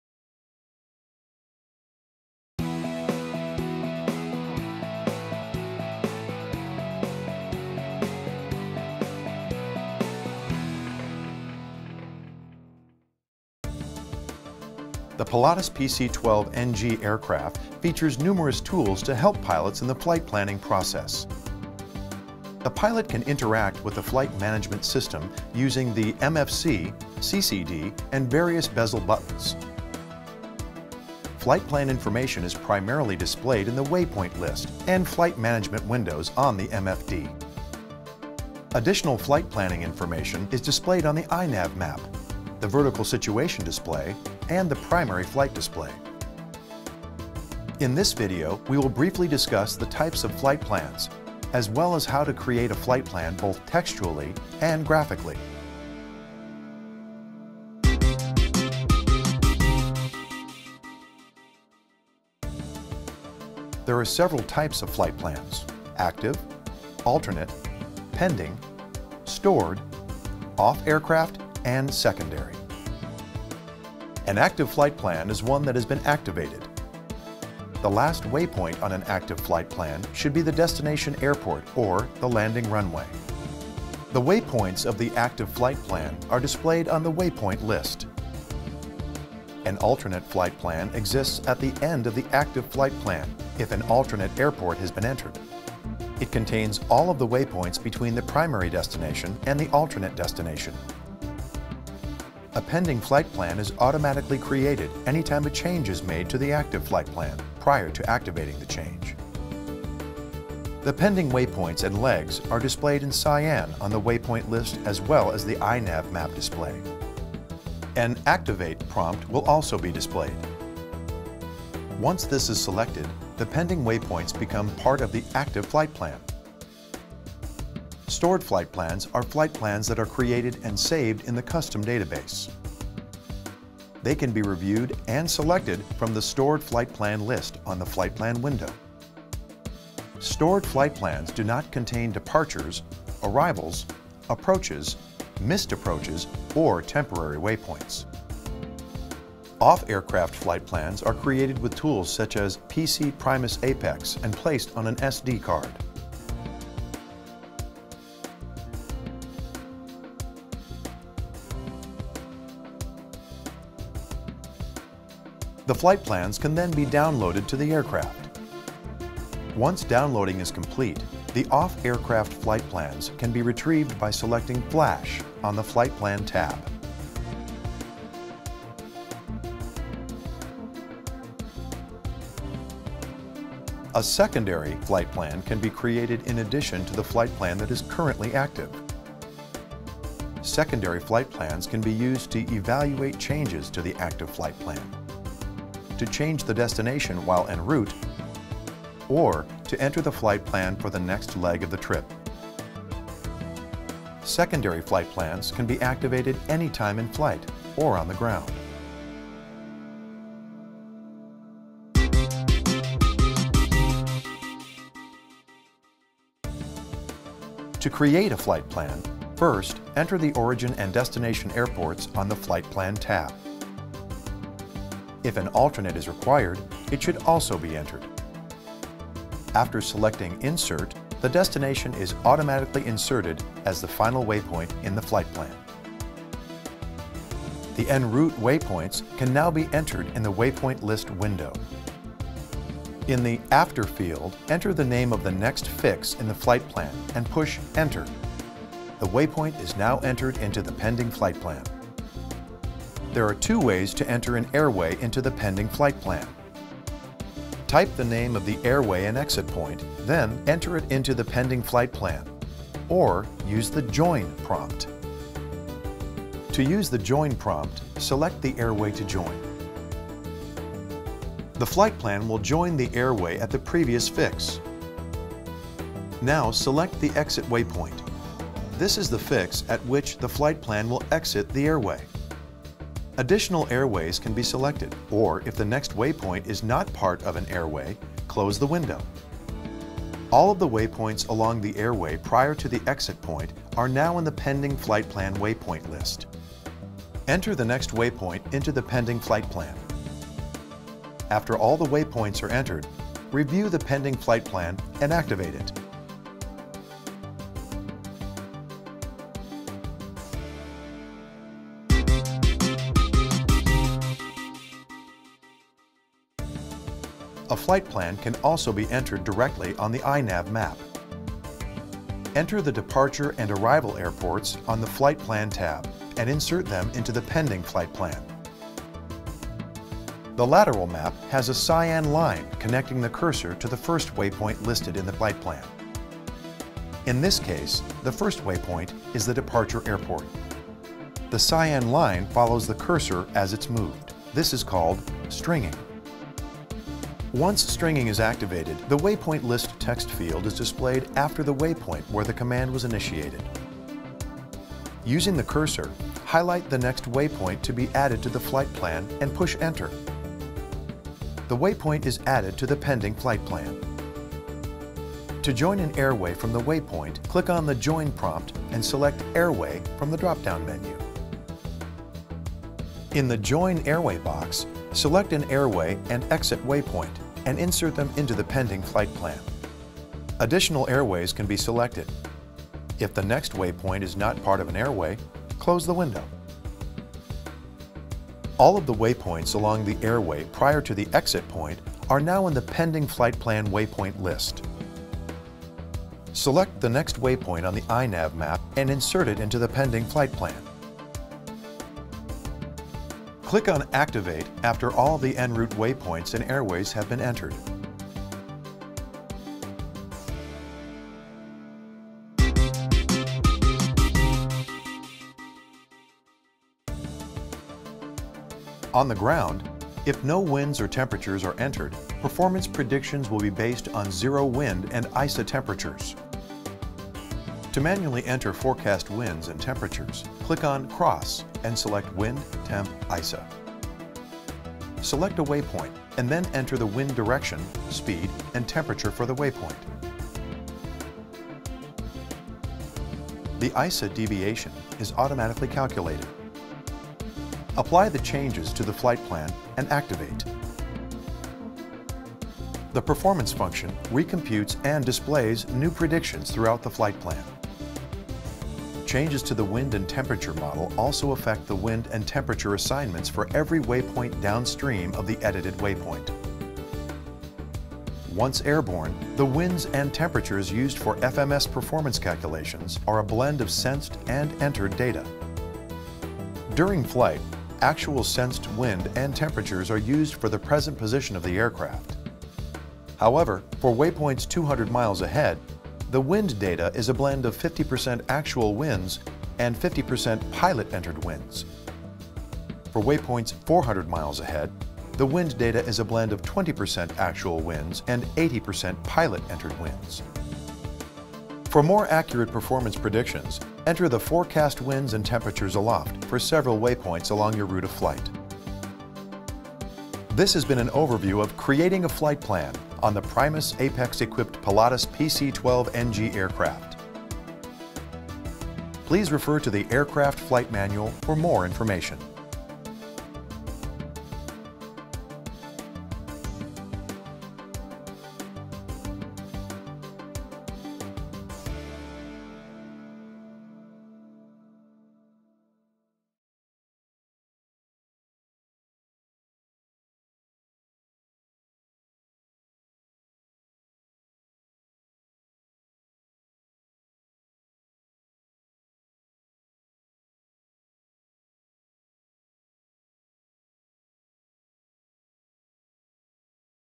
Honeywell Primus Apex® Pilatus PC-12 NG Creating a Flight Plan | Training | Honeywell Aerospace
In this video, we take a look at the Honeywell Primus Apex® Pilatus PC-12 NG aircraft and how to create a flight plan using the system.

The Primus Apex® is a cutting-edge cockpit management system that provides pilots with enhanced situational awareness and efficiency while in flight.

The Pilatus PC-12 NG aircraft features numerous tools to help pilots in the flight planning process. The pilot can interact with the flight management system using the MFC, CCD, and various bezel buttons. Flight plan information is primarily displayed in the waypoint list and flight management windows on the MFD. Watch this operational guide for more information.